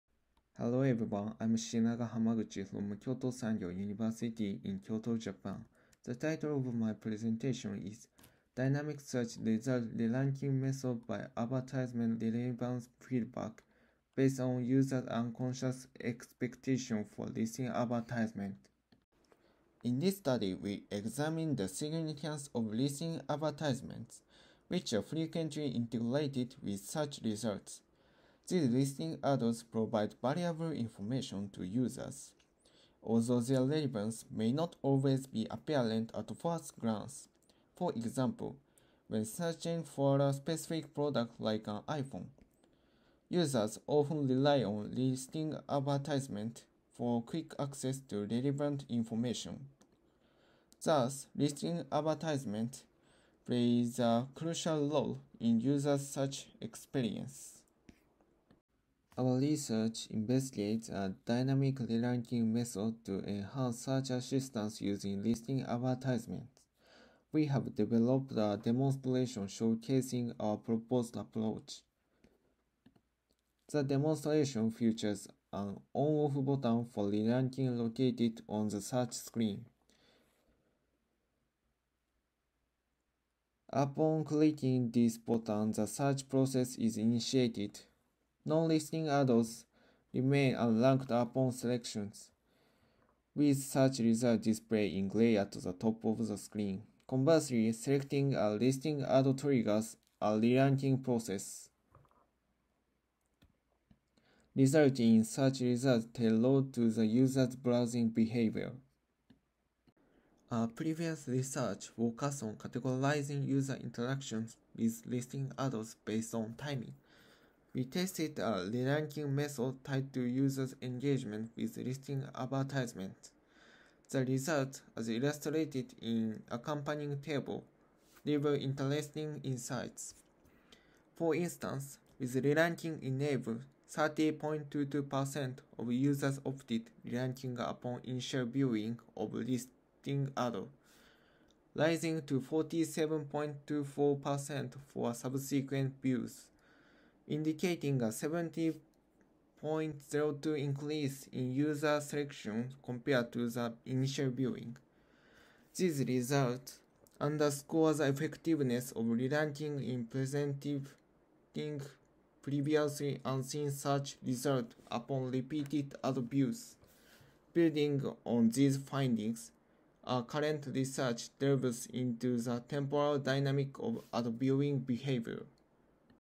[shp8181] Dynamic Search Results Re-ranking Method by Advertisement Relevance Feedback based on User
"Dynamic Search Results Re-ranking Method by Advertisement Relevance Feedback based on Users' Unconscious Expectations for Listing Advertisement

Da Li, Shigenaga Hamaguchi, Ruman Suyama, Shinsuke Nakajima and Yukiko Kawai"